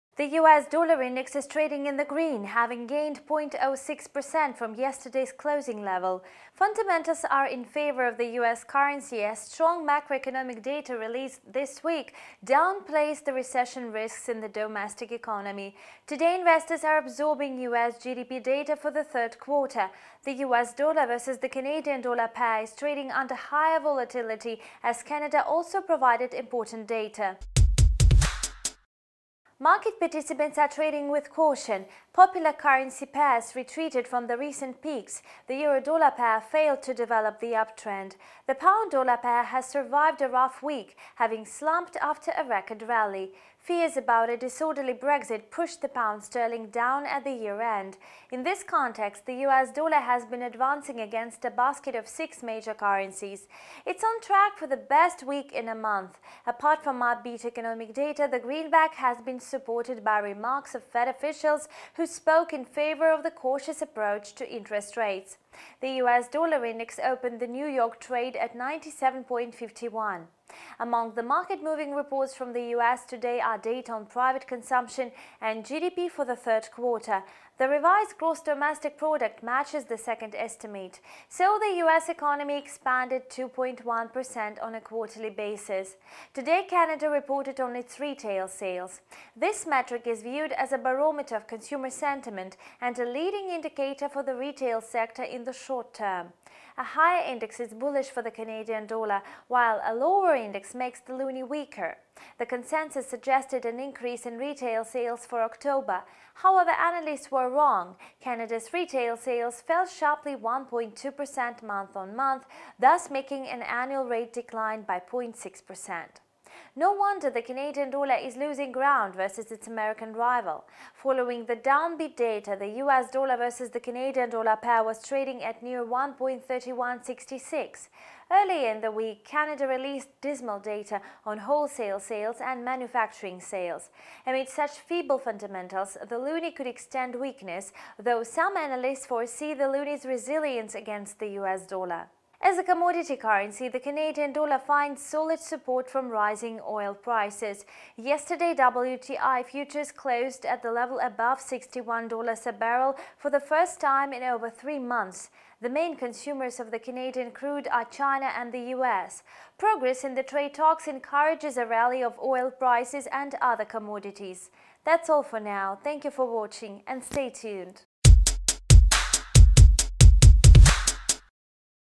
20.12.2019: USD picking up steam (USDХ, USD/CAD)
The US dollar index is trading in the green, having gained 0.06% from yesterday’s closing level. Fundamentals are in favor of the US currency as strong macroeconomic data released this week downplays the recession risks in the domestic economy. Today investors are absorbing US GDP data for the third quarter. The USD/CAD is trading under higher volatility as Canada also provided important data.
Market participants are trading with caution. Popular currency pairs retreated from the recent peaks. The euro/dollar pair failed to develop the uptrend. The pound/dollar pair has survived a rough week, having slumped after a record rally. Fears about a disorderly Brexit push the pound sterling down at the year end. In this context, the US dollar has been advancing against a basket of six major currencies. It is on track for the best week in a month. Apart from upbeat economic data, the greenback has been supported by remarks of Fed officials who spoke in favor of the cautious approach to interest rates. The US dollar index opened the New York trade at 97.51. Among the market-moving reports from the US today are data on private consumption and GDP for the third quarter. The revised gross domestic product matches the second estimate. So, the US economy expanded 2.1% on a quarterly basis.
Today Canada reported on its retail sales. This metric is viewed as a barometer of consumer sentiment and a leading indicator for the retail sector in the short term. A higher index is bullish for the Canadian dollar while a lower index makes the loonie weaker. The consensus suggested an increase in retail sales for October. However, analysts were wrong. Canada’s retail sales fell sharply 1.2% month-on-month, thus making an annual rate decline by 0.6%.
No wonder, the Canadian dollar is losing ground versus its American rival. Following the downbeat data, the USD/CAD pair was trading at near 1.3166. Earlier in the week, Canada released dismal data on wholesale sales and manufacturing sales. Amid such feeble fundamentals, the loonie could extend weakness, though some analysts foresee the loonie’s resilience against the US dollar.
As a commodity currency, the Canadian dollar finds solid support from rising oil prices. Yesterday, WTI futures closed at the level above 61 dollars a barrel for the first time in over three months. The main consumers of the Canadian crude are China and the US. Progress in the trade talks encourages a rally of oil prices and other commodities.